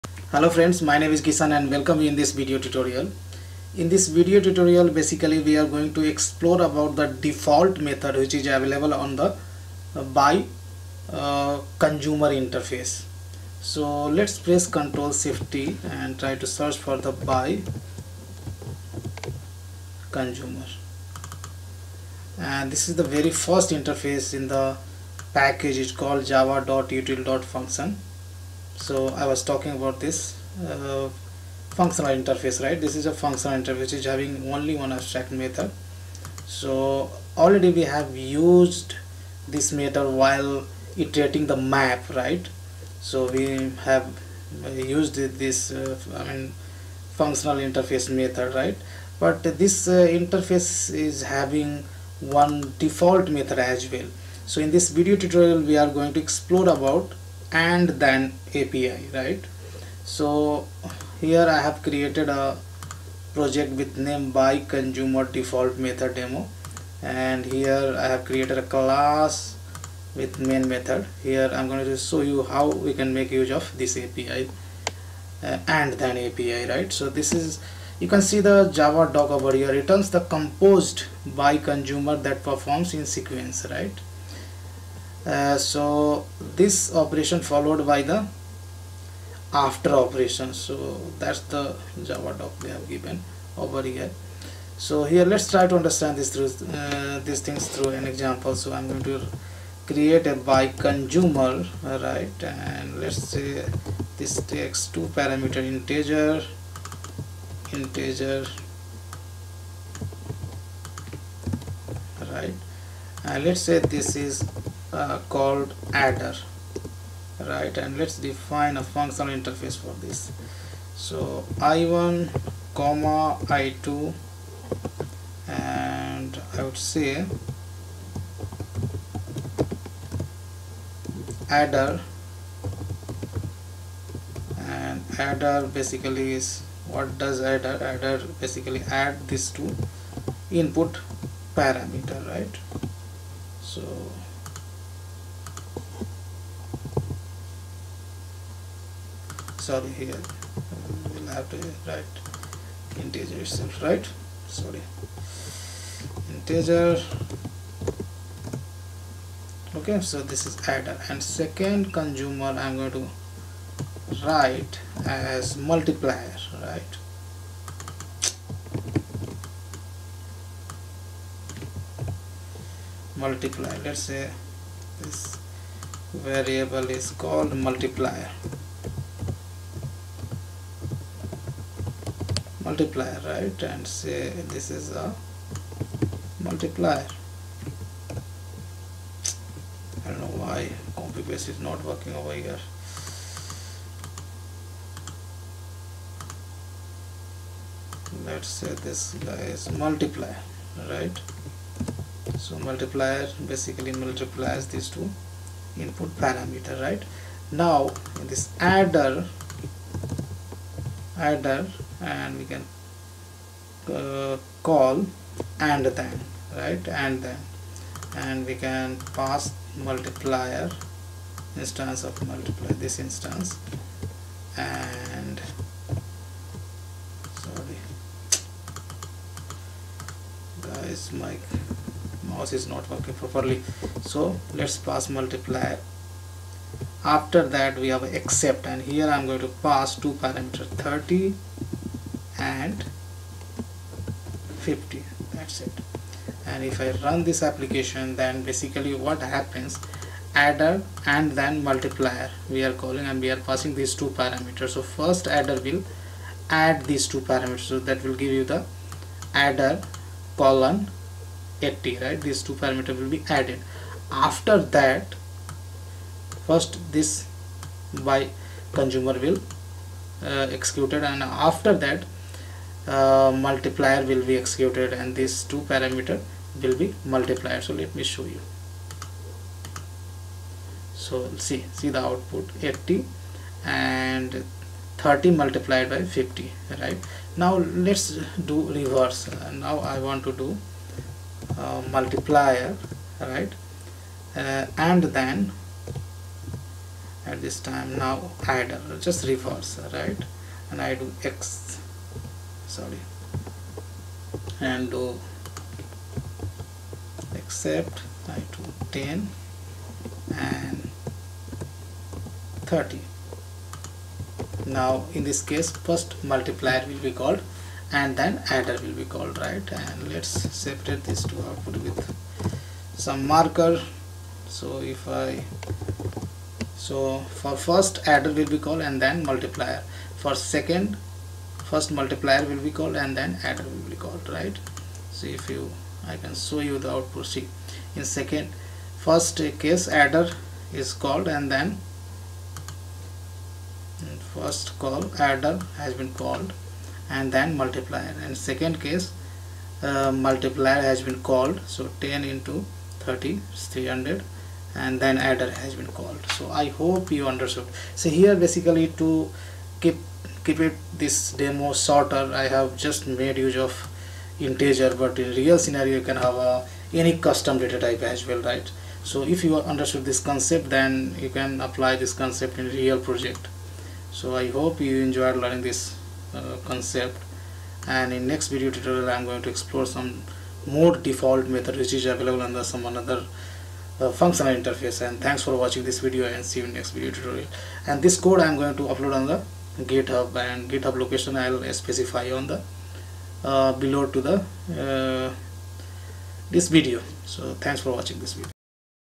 Java Lambda-BiConsumer andThen example
In this video tutorial you will learn about Java Lambda-BiConsumer andThen example using demo project
Below is the GitHub link to download source Code: